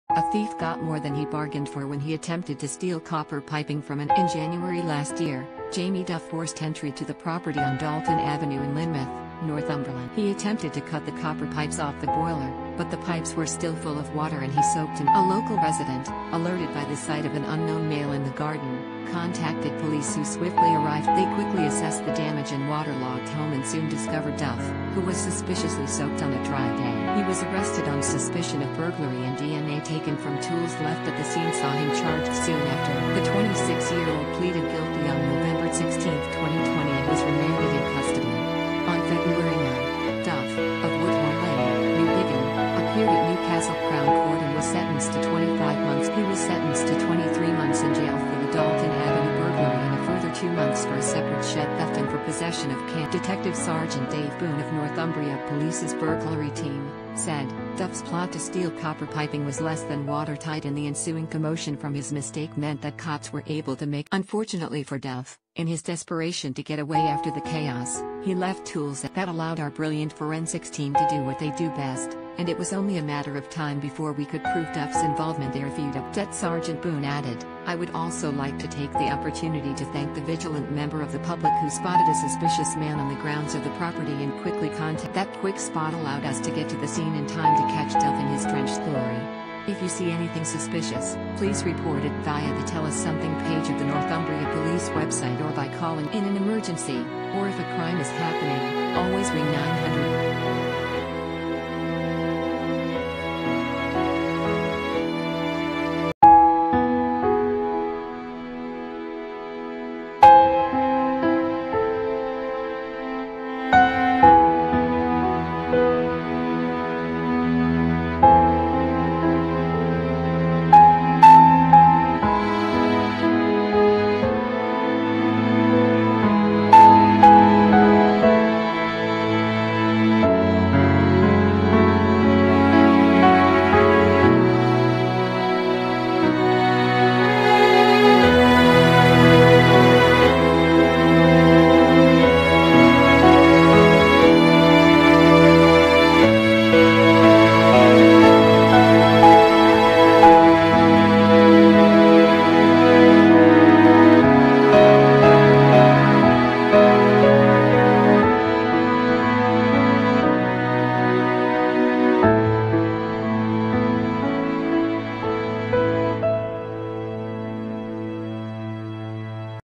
Thief left soaked after trying to steal copper piping from empty home
A thief got more than he bargained for when he attempted to steal copper piping from an empty house.  In January last year, Jamie Duff forced entry to the property on Dalton Avenue in Lynemouth, Northumberland.  He attempted to cut the copper pipes off the boiler, but the pipes were still full of water and he soaked himself.  A local resident, alerted by the sight of an unknown male in the garden, contacted police who swiftly arrived to investigate.  They quickly assessed the damage and water-logged home and soon discovered Duff, who was suspiciously soaked on a dry day.  He was arrested on suspicion of burglary and DNA taken from tools left at the scene saw him charged soon after.  The 26-year-old pleaded guilty on November 16 2020 and was remanded in custody.  On February 9, Duff, of Woodhorn Lane, Newbiggin, appeared at Newcastle Crown Court and was sentenced to 25 months behind bars.    He was sentenced to 23 months in jail for the Dalton Avenue burglary and a further two months for a separate shed theft and for possession of cannabis.  Detective Sgt Dave Boon of Northumbria Police’s Burglary Team, said: “Duff’s plot to steal copper piping was less than water tight and the ensuing commotion from his mistake meant that cops were able to make a swift arrest.  “Unfortunately for Duff, in his desperation to get away after the chaos, he left tools at the scene too.  “That allowed our brilliant forensics team to do what they do best – and it was only a matter of time before we could prove Duff’s involvement irrefutably.”   Det Sgt Boon added: “I would also like to take the opportunity to thank the vigilant member of the public who spotted a suspicious man on the grounds of the property and quickly contacted police.   "That quick spot allowed us to get to the scene in time to catch Duff in his drenched glory!”  If you see anything suspicious, please report it via the ‘Tell Us Something’ page of the Northumbria Police website or by calling 101.   In an emergency, or if a crime is happening, always ring 999.